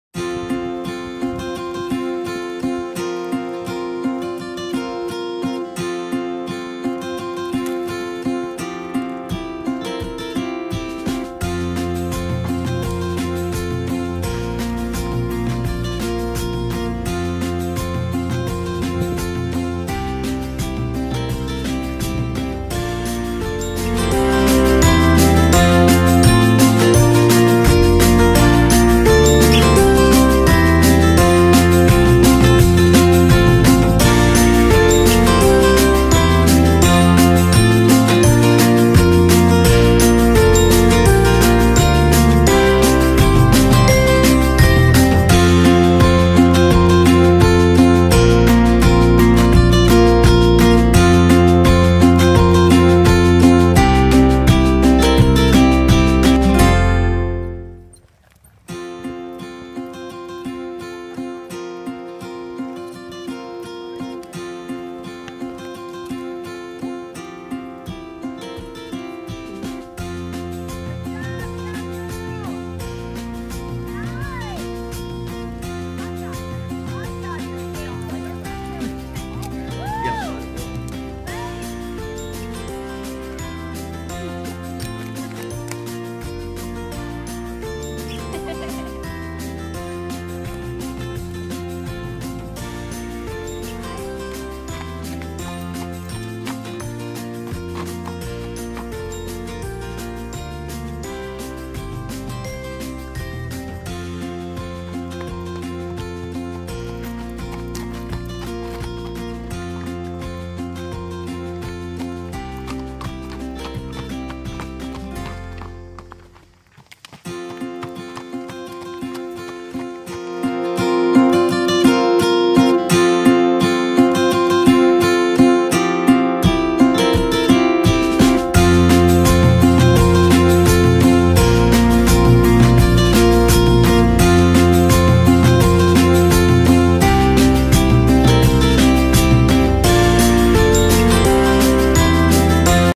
***FOR SALE***
C Dazzling Image "Indigo"
Fancy 15.1 hand, 6 Y/O Black AQHA mare. Big bodied, and Versatile with an English/Western background. Experienced in 2'6"/2'9" Jumpers and local hunters. She loves sorting, pole bending and barrel racing. She has been ridden in 4-H and PonyClub programs. She has done lots of trail rides and Hunter Paces. I would recommend this horse to someone who either wants to continue training her english or western. She's very sweet and loves attention. Only for sale as outgrown. Perfect for anyone who wants to finish her they're way. 6,000 obo